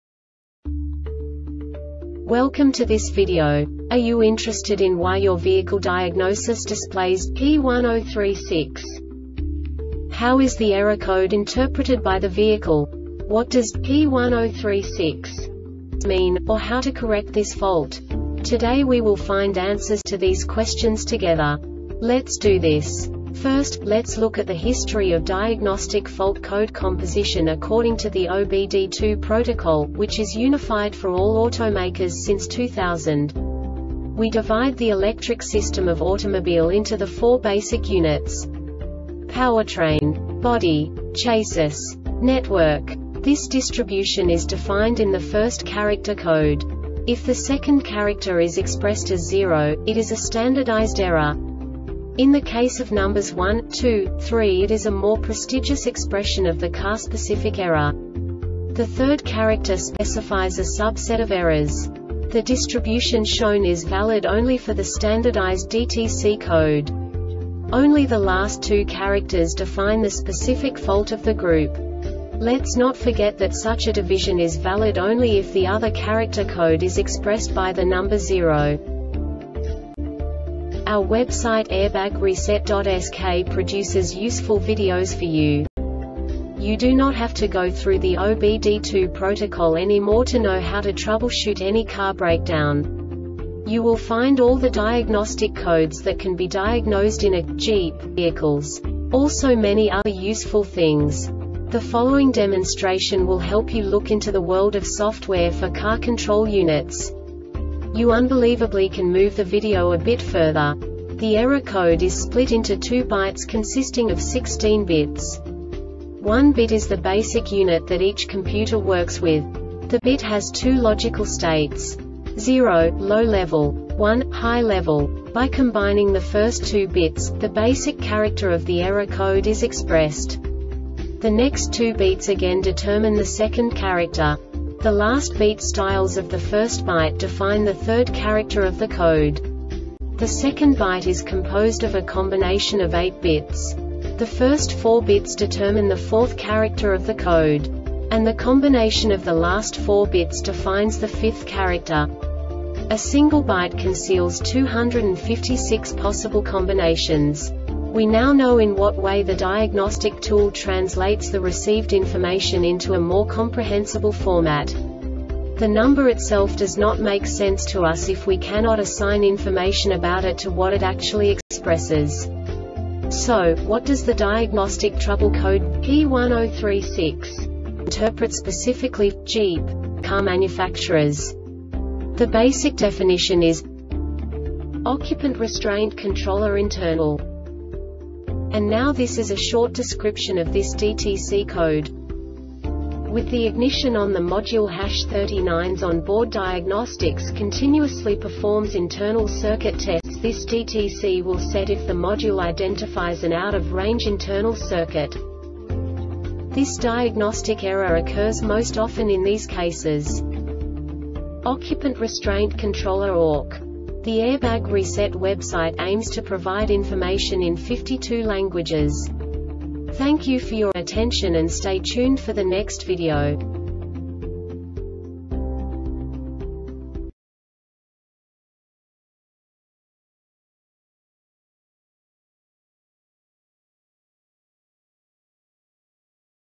DTC Jeep P1036 Short Explanation
Contents:
0:21 Basic DTC analysis according to OBD2 protocol standard.
1:48 Insight into programming
2:58 A diagnostically understandable description of the Diagnostic Trouble Code
3:05 Basic error definition

Make:
Jeep
Code:
P1036
Definition:
OCCUPANT RESTRAINT CONTROLLER INTERNAL 4
Description:
With the ignition on. The module&#39;s on board diagnostics continuously performs internal circuit tests. This DTC will set if the module identifies an out of range internal circuit
Cause: